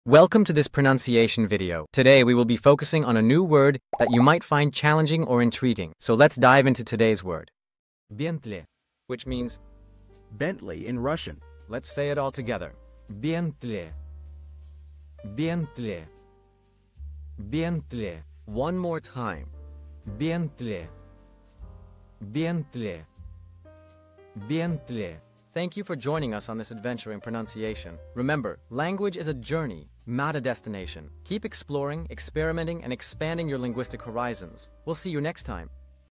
How to pronounce Bentli | Бентли (Bentley in Russian)
Master the Pronunciation of 'Bentli | Бентли' - which means : Bentley in Russian 📝 with @PronunciationRussian 🎓🗣️ - [Your Guide to Russian]

Learn to pronounce 'Bentli' perfectly in Russian with our easy and engaging tutorial by @PronunciationRussian! 📖📽️ In this comprehensive video, we provide you with native speaker examples 🗣️👂 and phonetic breakdowns 🎵 to ensure you get the pronunciation just right.